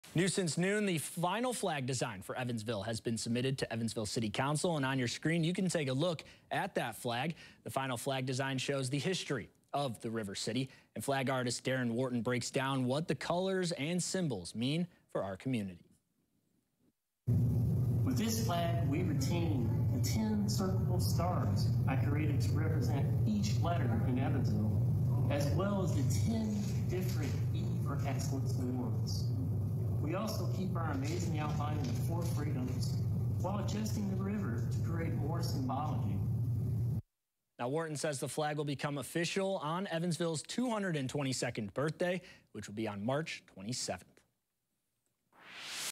Final Evansville Flag design submitted to city council
The final flag design by artist Darren Wharton, for Evansville's new city flag, has been submitted to Evansville city council.